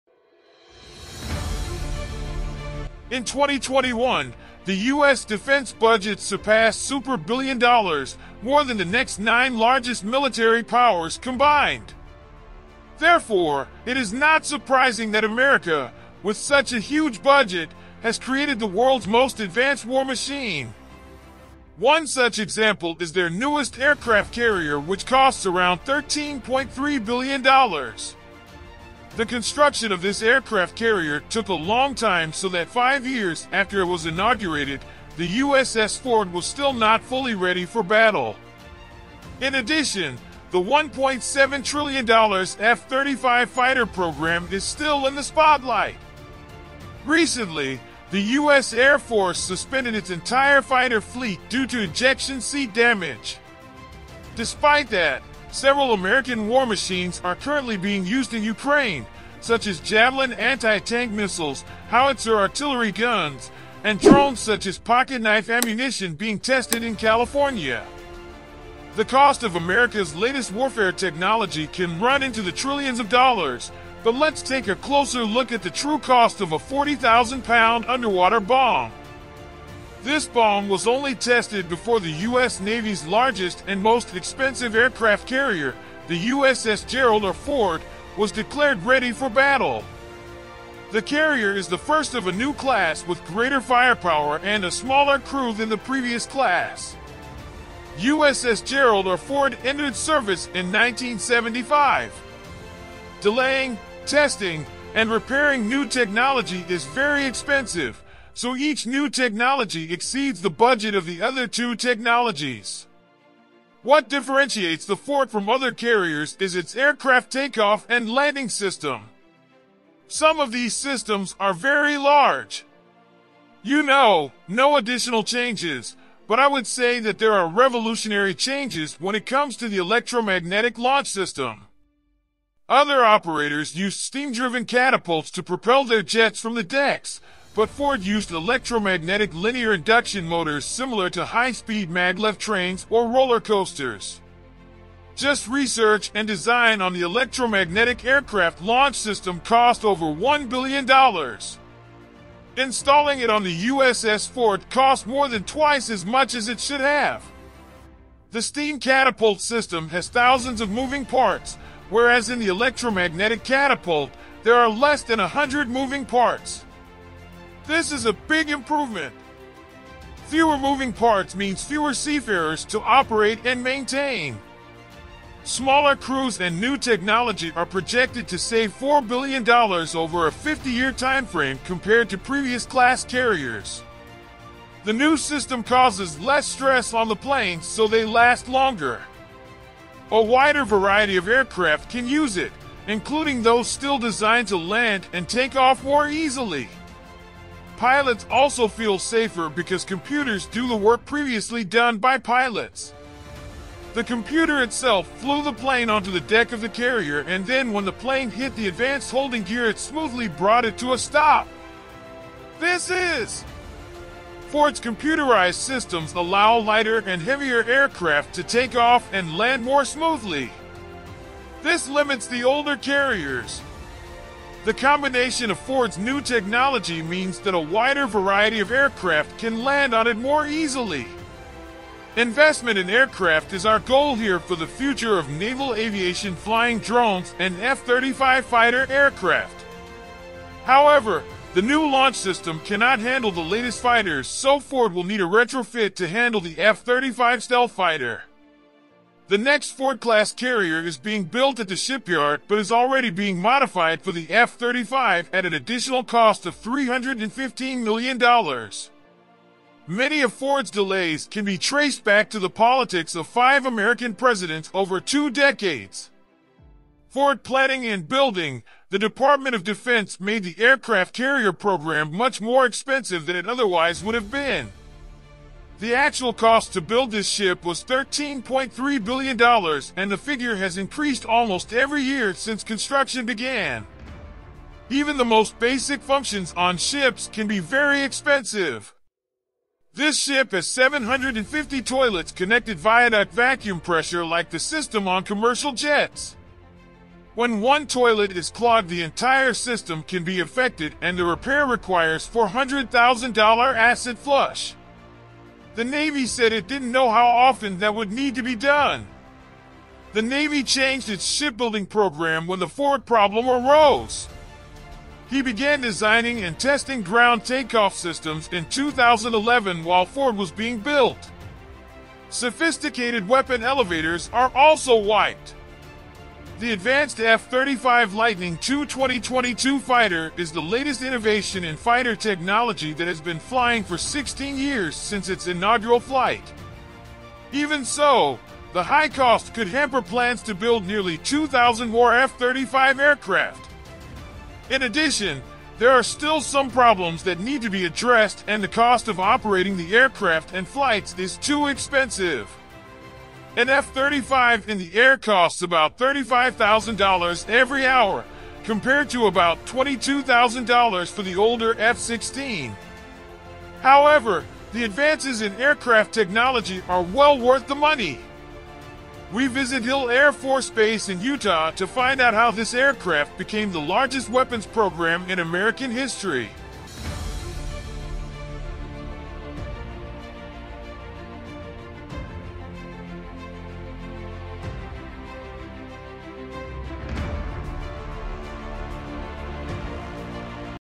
How the U S Military Spending Super Billion Dollars a Year on War Machines Actual Cost Business I
In 2021, the US defense budget will pass $800 billion, more than the next nine largest military powers combined. Therefore, it is not surprising that America, with such a huge budget, has created the world's most advanced war machine. One such example is their newest aircraft carrier which costs around 13.3 billion dollars. The construction of this aircraft carrier took a long time so that five years after it was inaugurated, the USS Ford was still not fully ready for battle.